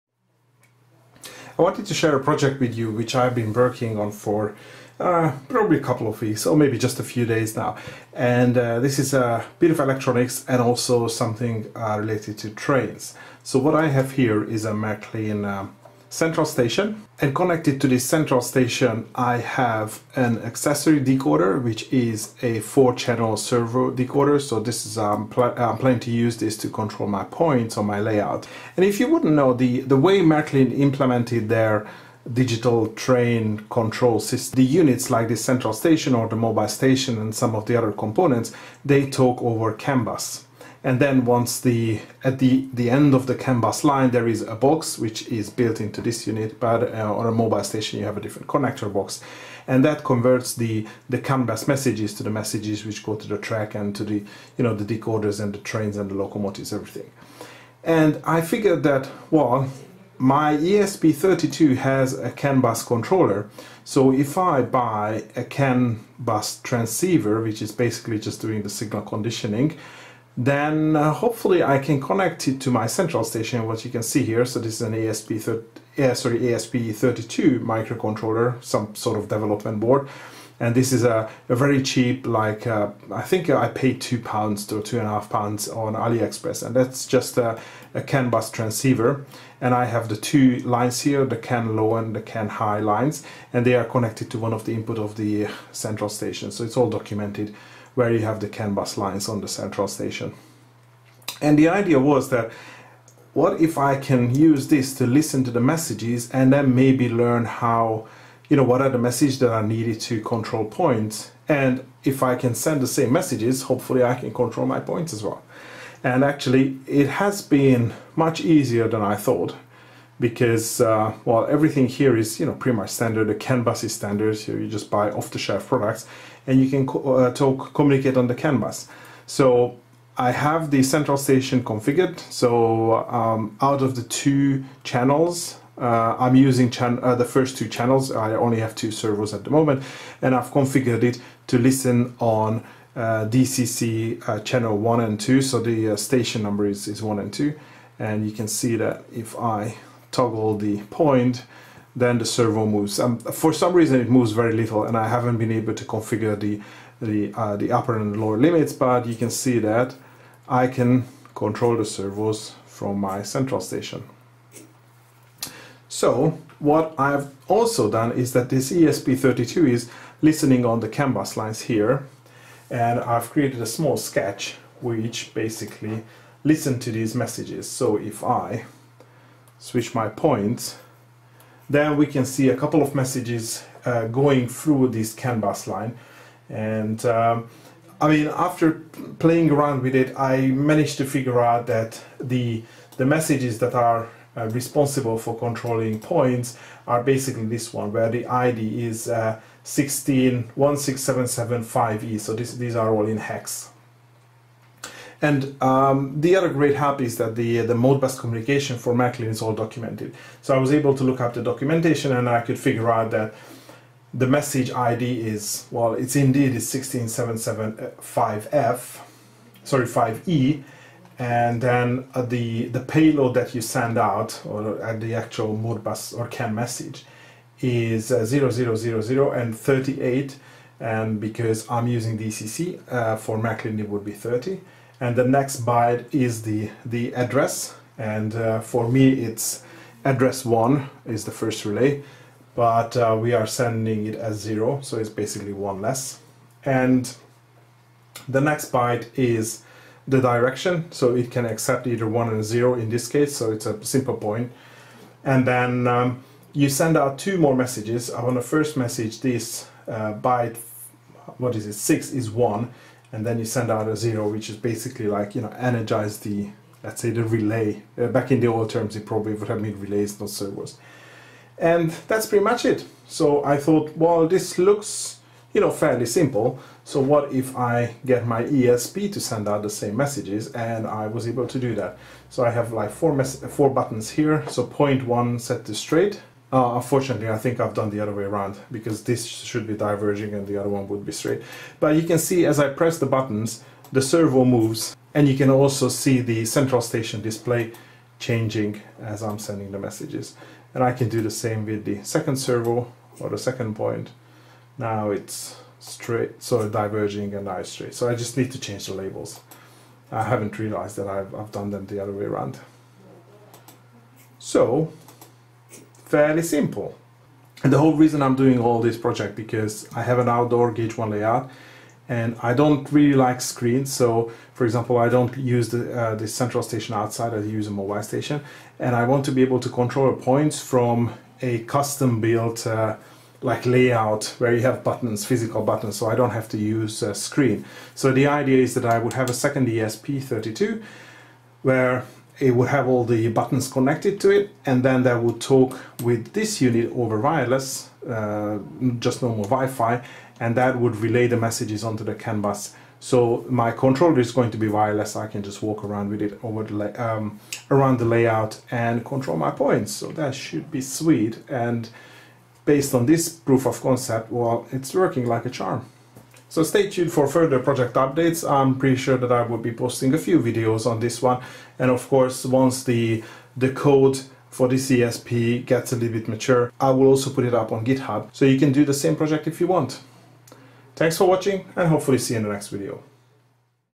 Marklin CAN-Wifi Gateway is born
As an end goal I would like to build a mobile point (switch) control board to control my points around my layout. I want a physical switches not a screen and it would be great if it would be wireless.

In this video I show how I start controlling DCC accessories over the CAN bus of a Marklin digital control system. Here I am using a Central Station 2 and a ESP32 with a CAN transceiver module.